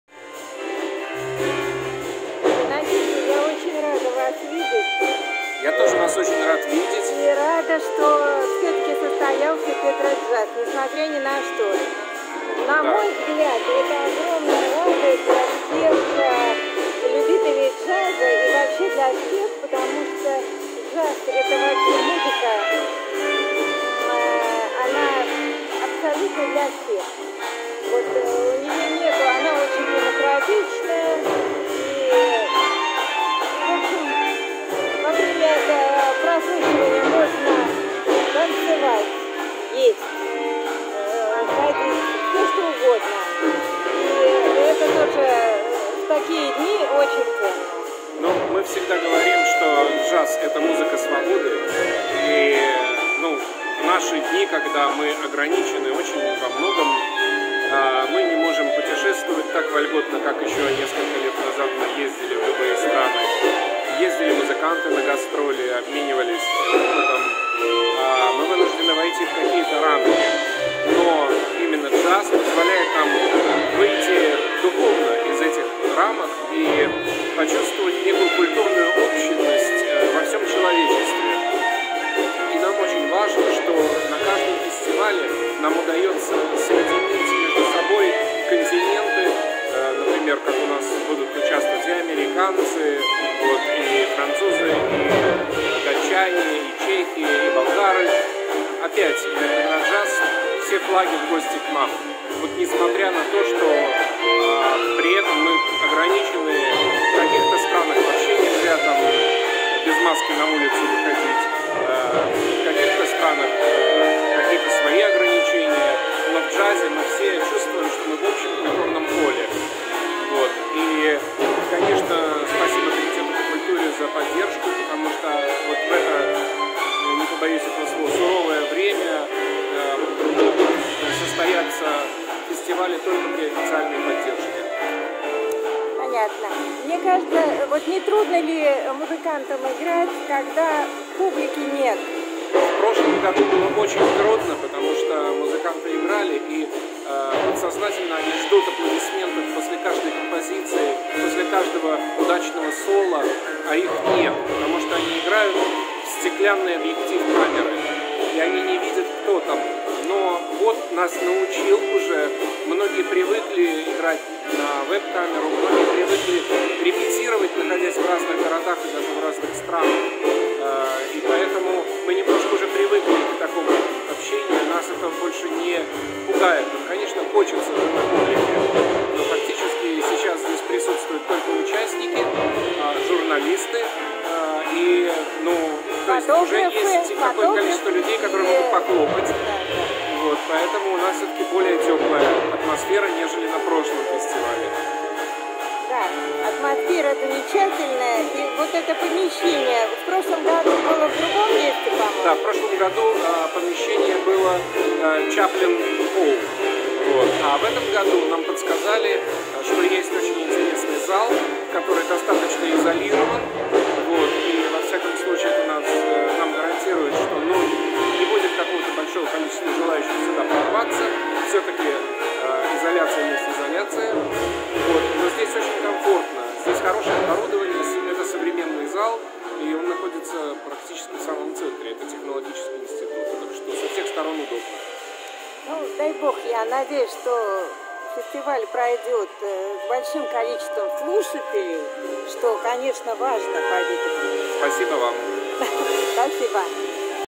Иннокентий Волкоморов о фестивале
5 ноября XVII Международный джазовый фестиваль ПЕТРОДЖАЗ стартовал в режиме online. Фестиваль пройдёт в течение 3 дней. Начало в 17 часов. 5 разных коллективов, каждый в течение 45 минут на сцене, в перерывах идёт интернет-включение зарубежных групп. После выступления Иннокентий Волкоморов записывает интервью с отыгравшей группой в фойе на первом этаже. Как всегда представлены все стили импровизационной музыки: бибоп и блюз, фанк и клезмер, рок-н-ролл и этно. Музыканты из России, США, Чехии, Дании, Швейцарии, Латвии, Франции и Армении с 5 по 7 ноября в режиме онлайн при поддержке Комитета по культуре СПб с 17 до 22, включайте трансляцию вконтакте!